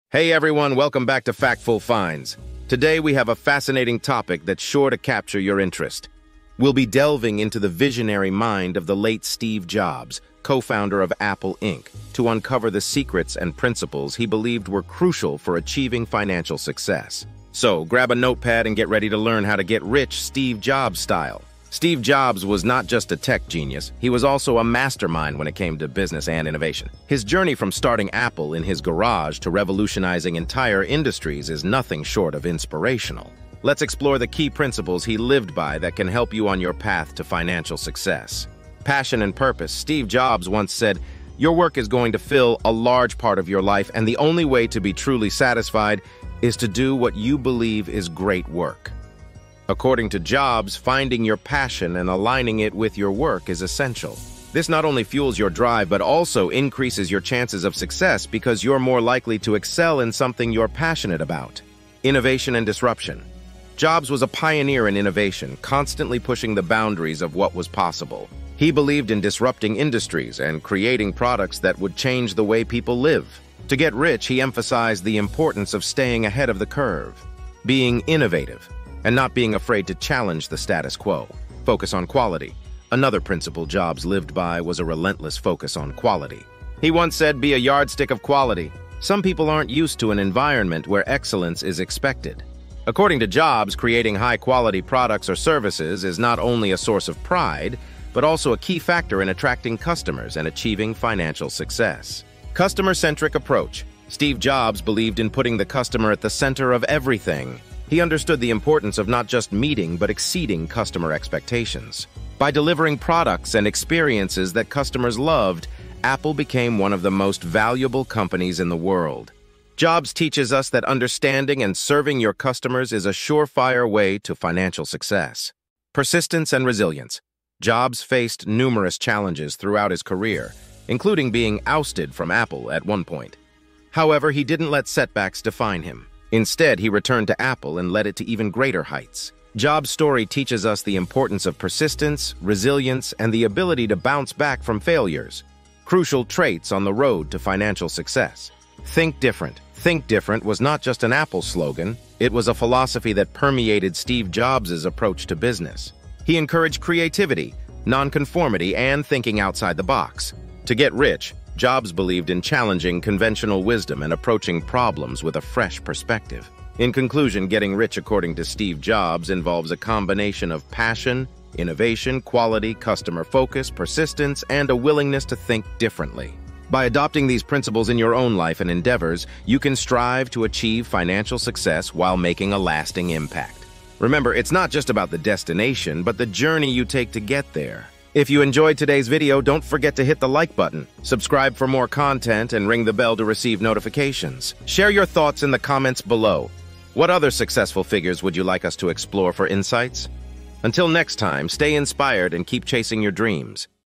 6 Ways To Get Rich According To Steve Jobs
Discover the keys to monetary prosperity with advice from the renowned Steve Jobs! 🚀 Come explore the ideas that drove Jobs as he established Apple Inc. from the ground up with us on FactFul Finds. See how your own success may be paved with passion, creativity, quality, and customer attention. Are you prepared to adopt a new way of thinking? Now let's get going! 💡💰